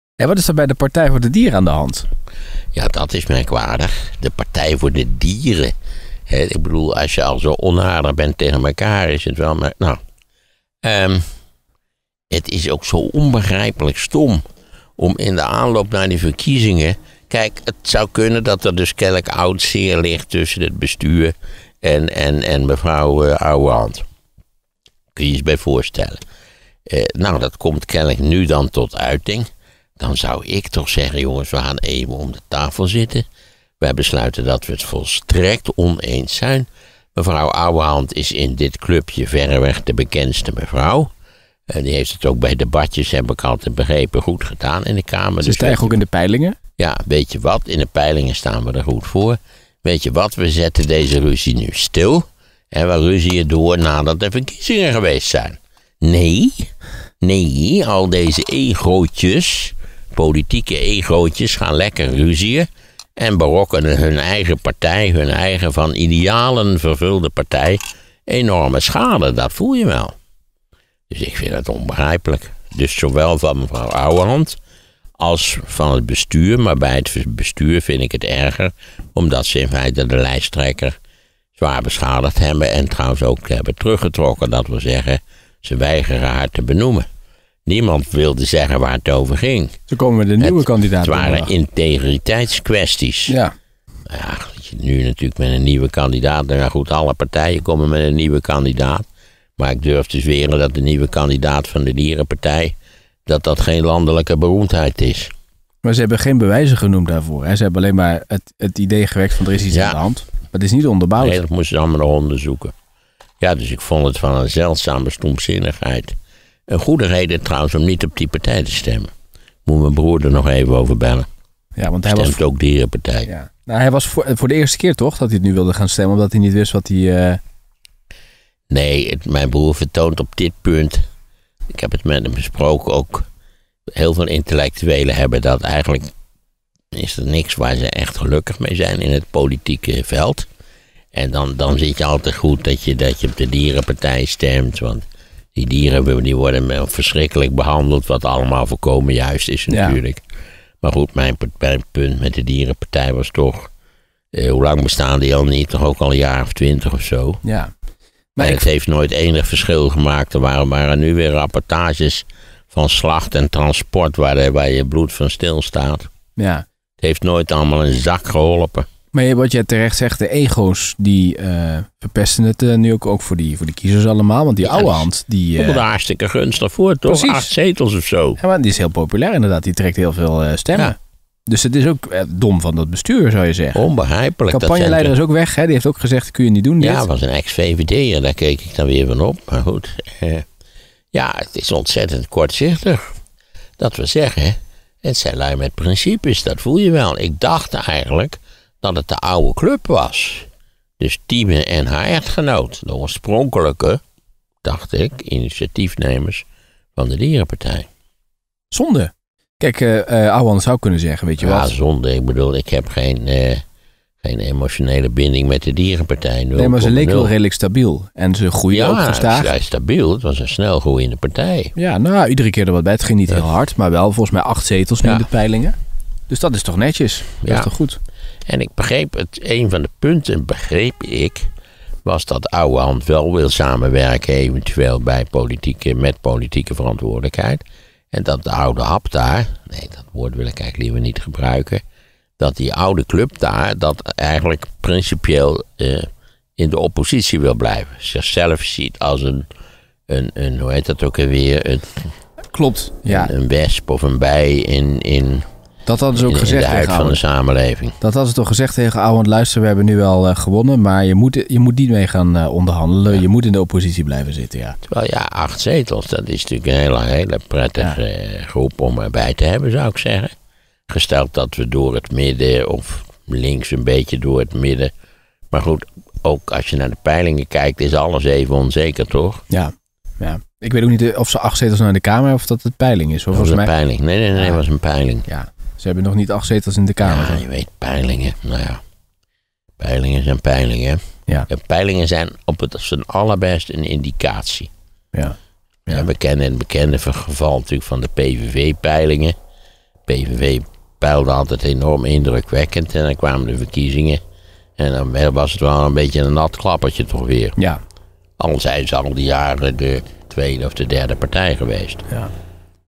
Bom ontploft bij Partij voor de Dieren: 'Onbegrijpelijk stom!'
De chaos is compleet bij de Partij voor de Dieren. Esther Ouwehand was gedumpt als partijleider, maar is weer terug. Het bestuur is vertrokken.

Maarten van Rossem reageert op de ontstane commotie in gesprek met presentator Tom Jessen.

▬ SHOWNOTES  ▬▬▬▬▬▬▬▬▬▬

▬ MEER MAARTEN  ▬▬▬▬▬▬▬▬▬▬

▬ LUISTERBOEKEN  ▬▬▬▬▬▬▬▬▬▬

▬ SOCIALS  ▬▬▬▬▬▬▬▬▬▬